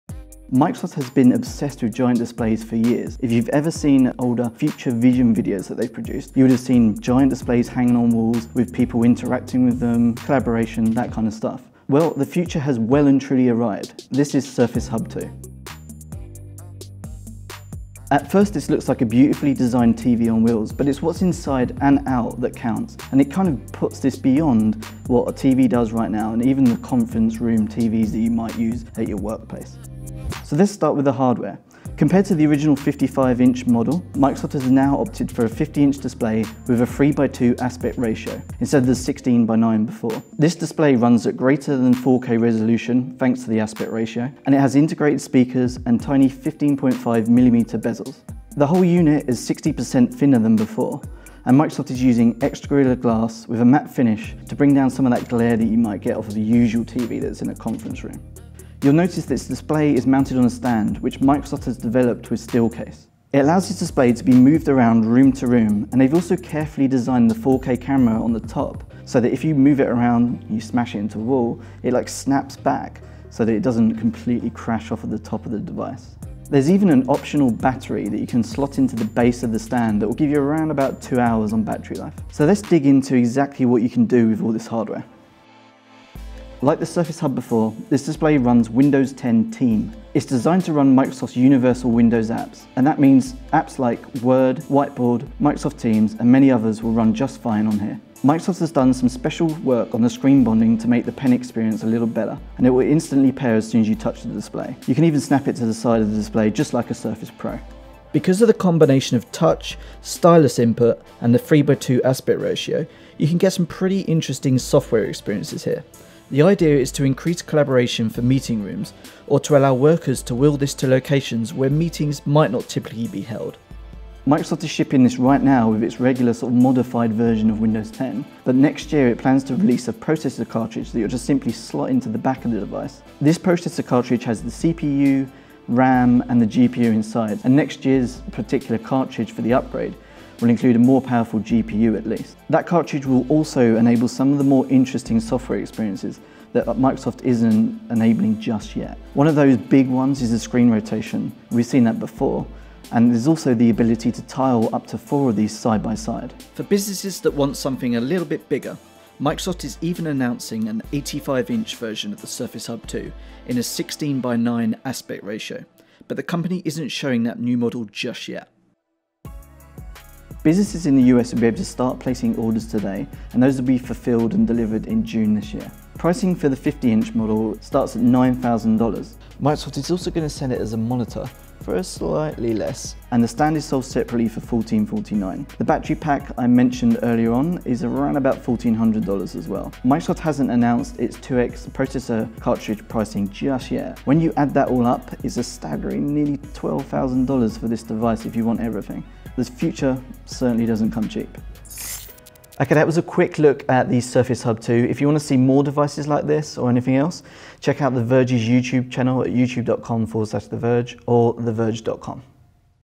Microsoft Surface Hub 2 hands-on: a $9K PC on wheels
With a new 50-inch 3:2 display, integrated speakers, and tiny 15.5 millimeters bezels, the Surface Hub 2S is a beautifully designed PC on wheels. Hub 2S is meant to live in your workspace and runs Windows 10 Team with support for apps like Word, Whiteboard, Skype and Microsoft Teams. The company plans to release a Surface Hub 2X cartridge next year that will have more powerful hardware to enable a lot more interesting software experiences like screen rotation and tiling multiple Hub2 devices side by side. Tom Warren visits Microsoft for a hands-on preview of the new Hub 2S which ships this June starting at $8,999.